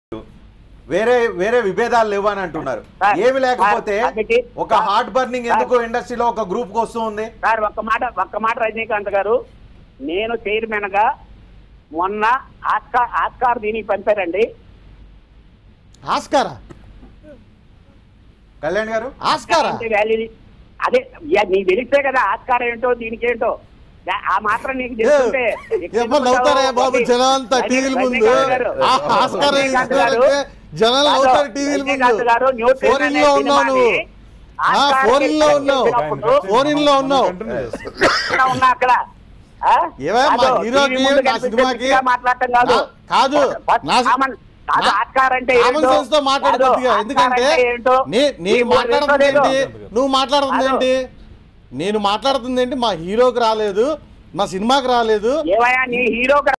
Nandi awards funny conversation
When c. Kalyan says he sent the movies list to “OSCAR” look at the rajnikanth expression and funny side of nallamalupu bujji